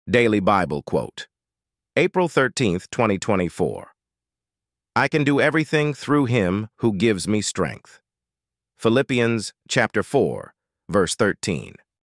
Philippians 4:13
Bible quote short of the day - April 13, 2024 | Philippians 4:13
I can do everything through him who gives me strength. [NIV]
I can do all things through Christ who strengthens me. [NKJV]
I can do all things through him who strengthens me. [ESV]
For I can do everything through Christ, who gives me strength. [NLT]
 I am able to do all things through him who strengthens me. [CSB]